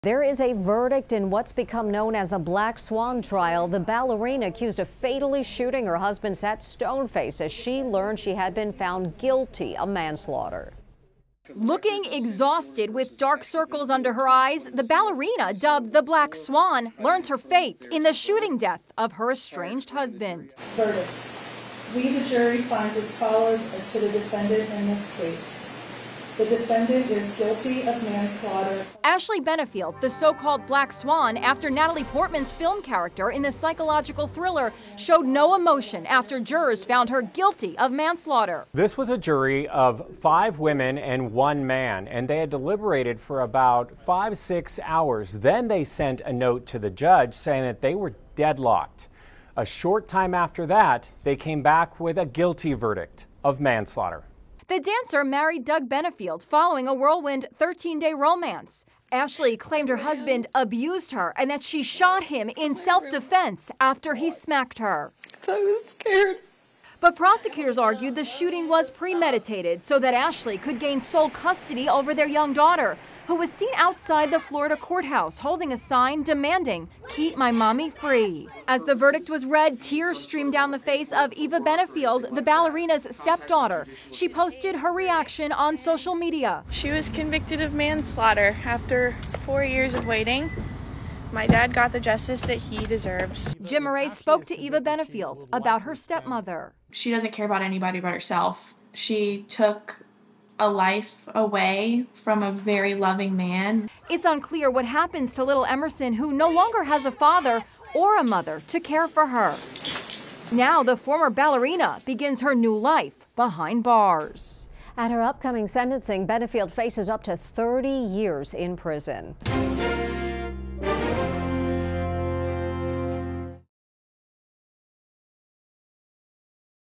Former Ballerina Convicted of Manslaughter in Death of Husband
Former ballerina Ashley Benefield was convicted of manslaughter over the shooting death of her estranged husband. Benefield claimed her husband abused her and that she shot him in self-defense after he smacked her. Prosecutors argued the shooting was premeditated so that she could gain sole custody over their young daughter. A sentencing hearing will be scheduled at a later date. Benefield faces up to 30 years in prison.